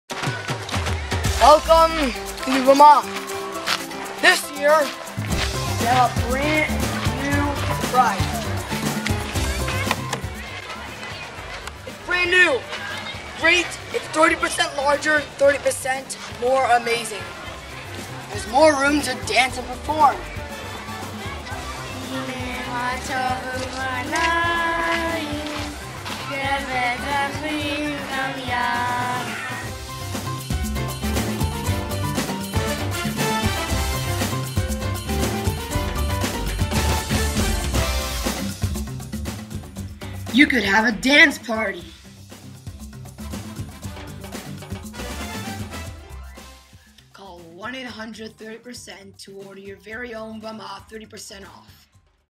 Camp Ad - K'firim: 30% Larger במה
A Video by the K'firim edah.
Created as part of the Camp Ad project in the Video Chug.
Camp Ramah in Canada - Summer 2011, Session 1.